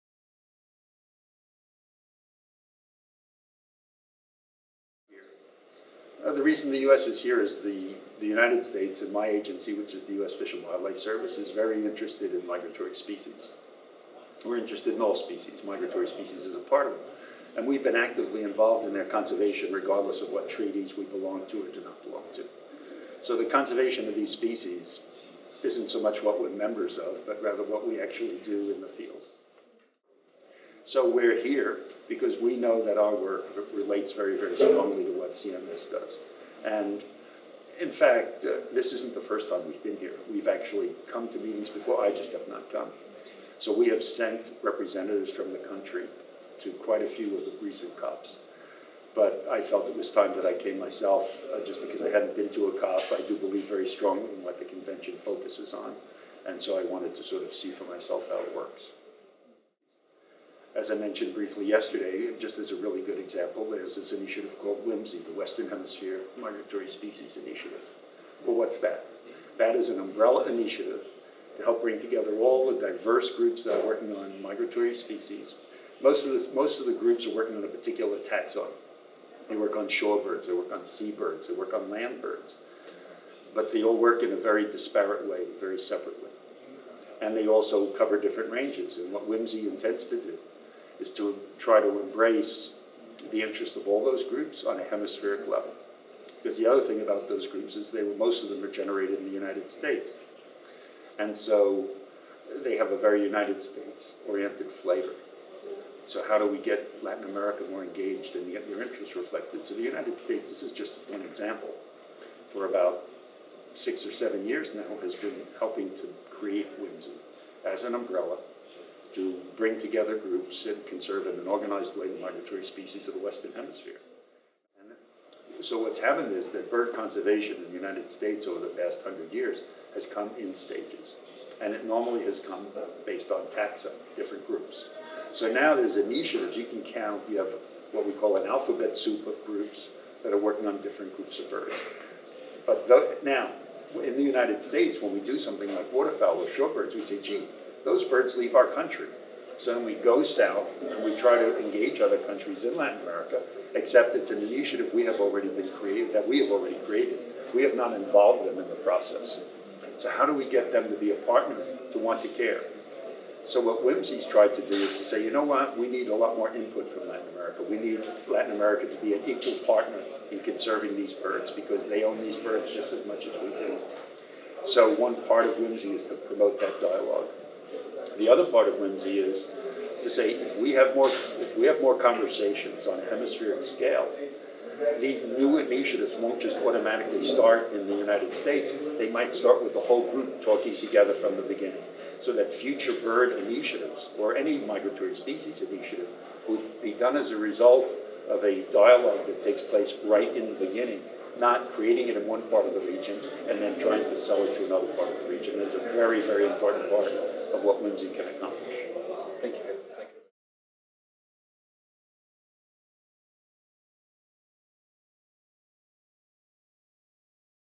CMS COP10 Interview with Herbert Raffaele
CMS COP10 Interview with Herbert Raffaele, Chief, Division of International Conservation, U.S. Fish & Wildlife Service